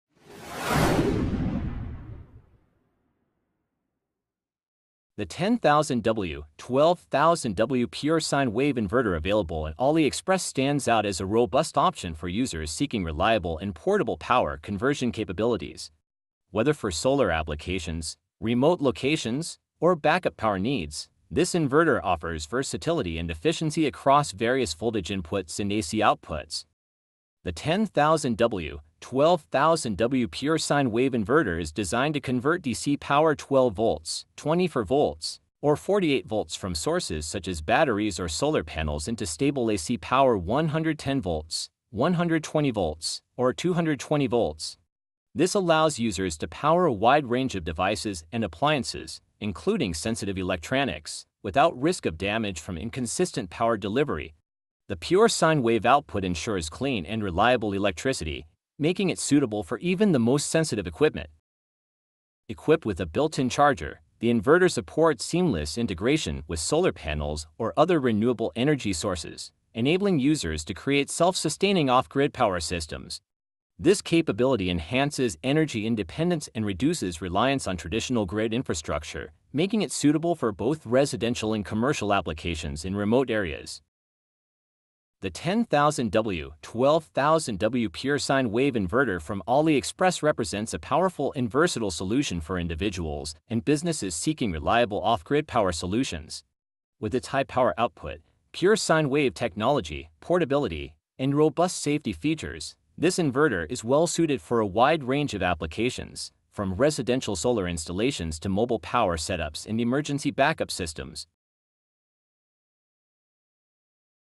Pure Sine Wave Inverter Portable Charger Solar Review Aliexpress
Specs 10000W 12000W Pure Sine Wave Inverter 10KW DC 12V 24V 48V To AC 110V 120V 220V Portable Charger Solar Off Grid Power Inverters Review Aliexpress, The 10000W 12000W Pure Sine Wave Inverter is a powerful device designed for off-grid solar power systems. It converts DC 12V, 24V, or 48V to AC 110V, 120V, or 220V, providing reliable power for various applications. This inverter features a pure sine wave output, ensuring compatibility with sensitive electronics and appliances. It is equipped with a portable charger, making it ideal for remote locations and emergency power needs. Perfect for both residential and commercial use, this inverter supports efficient and stable energy conversion for off-grid living.